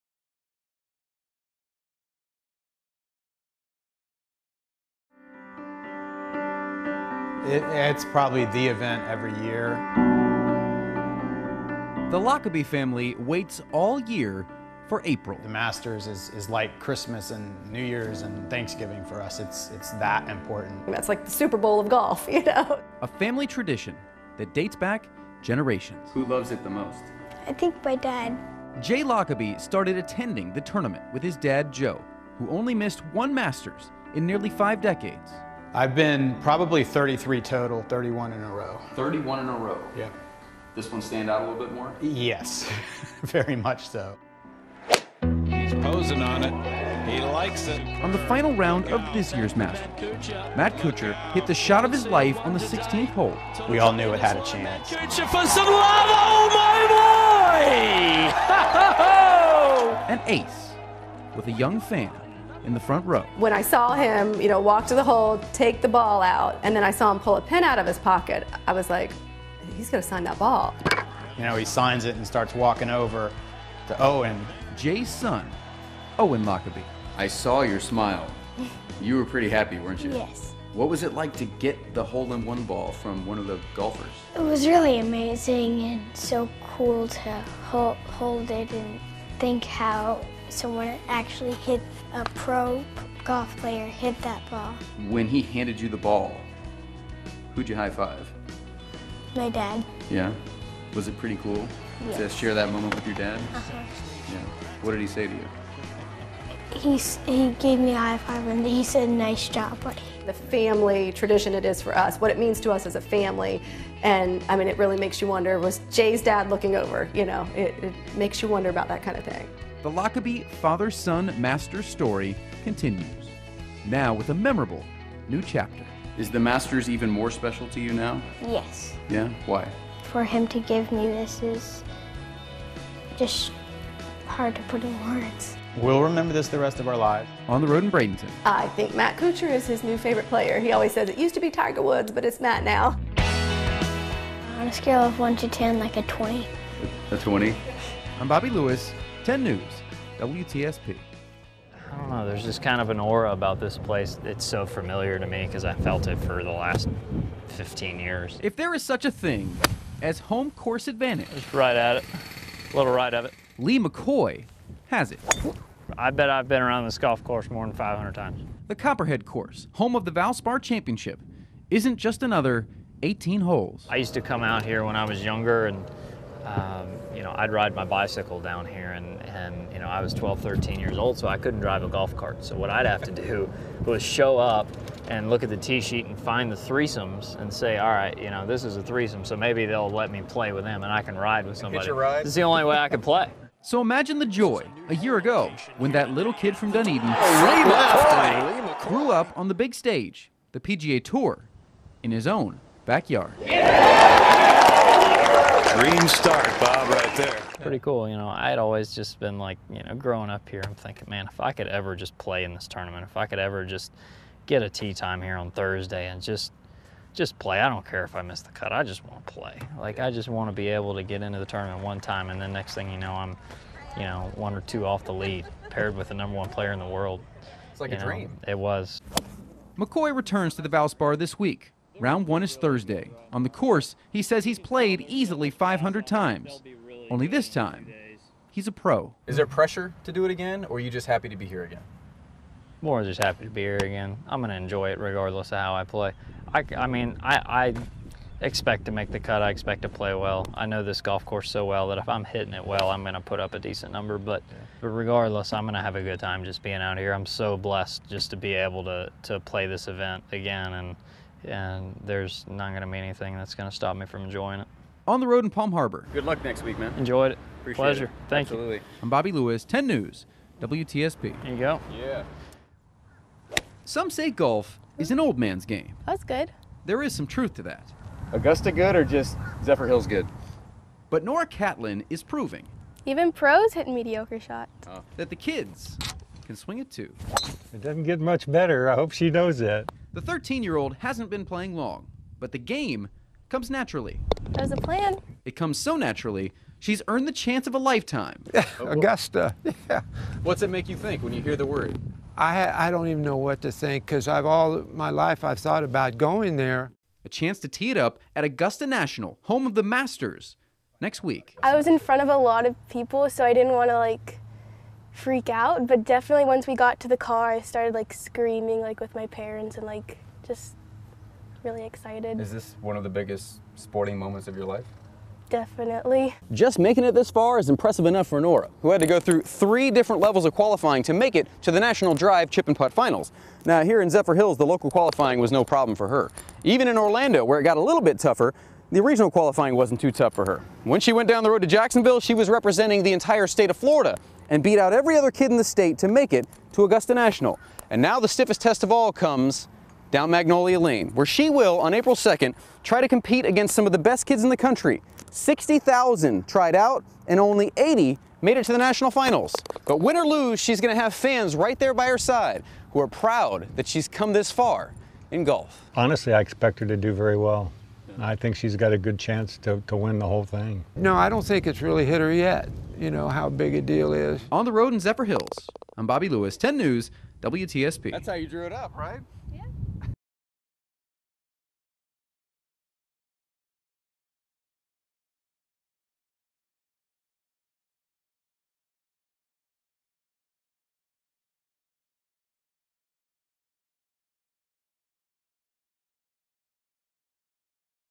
2017 GOLF REPORTING
Collection of golf-related sample work from Bobby Lewis, Tampa Bay-based reporter for WTSP-TV (CBS).